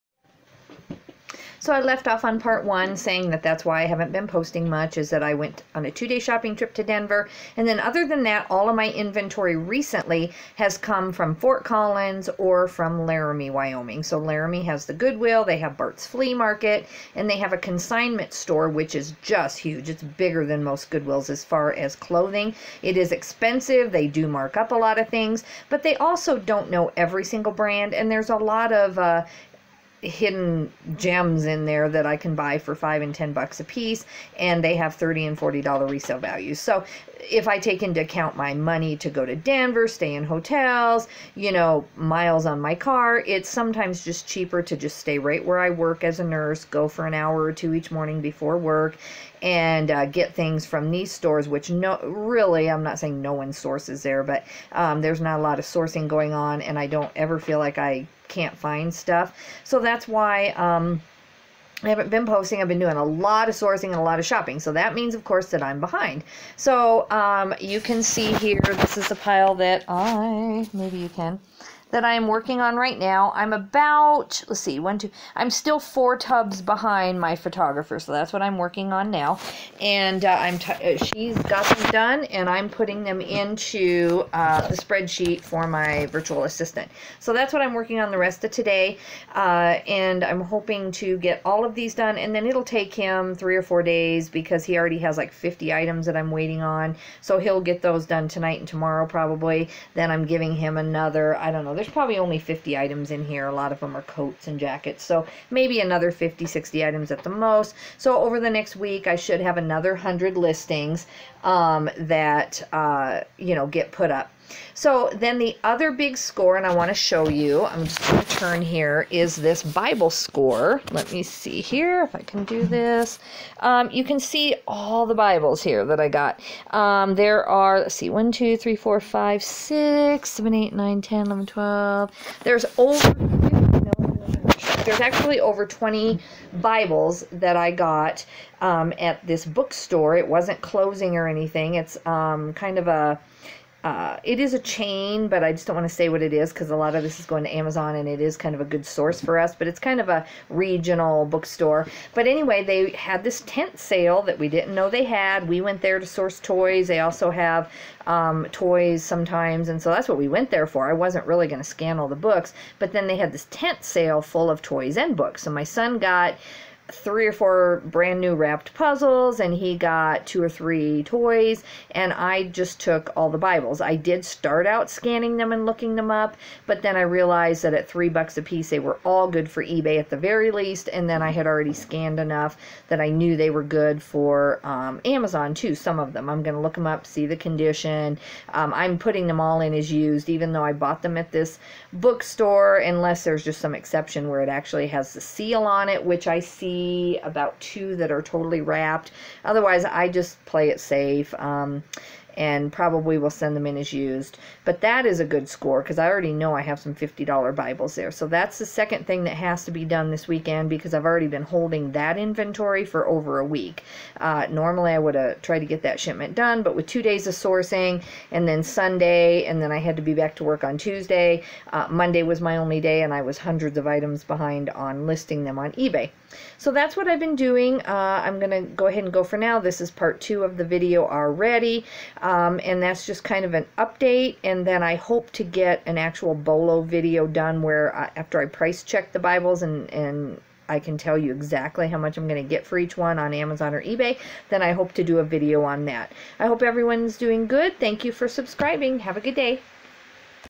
part 2 September update video. Tons of Sourcing. Still behind my photographer as far as listing
Part 2: Huge Bible haul; Still behind my photographer as far as listing.